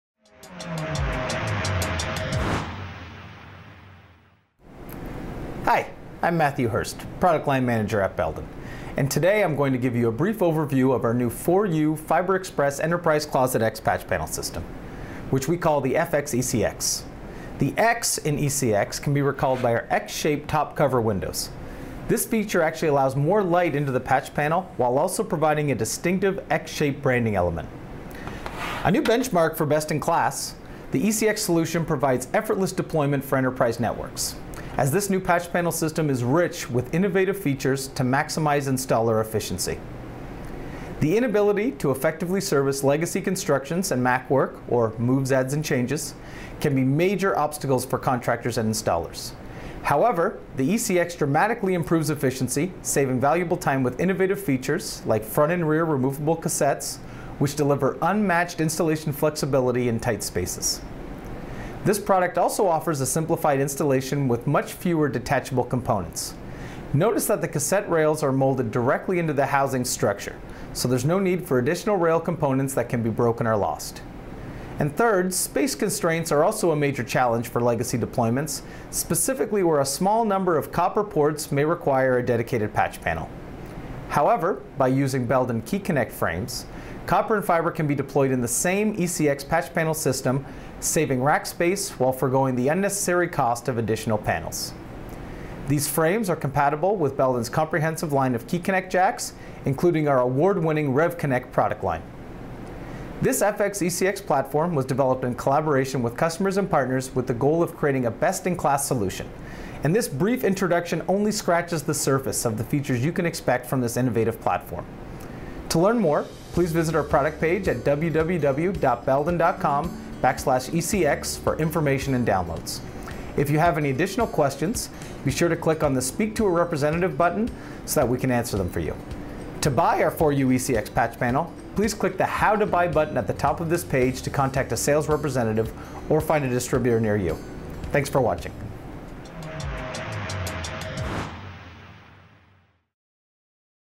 FX ECX 4U Overview | Belden
In this video, Product Line Manager Matt Hurst gives a quick overview of the Belden 4U ECX LAN Housing.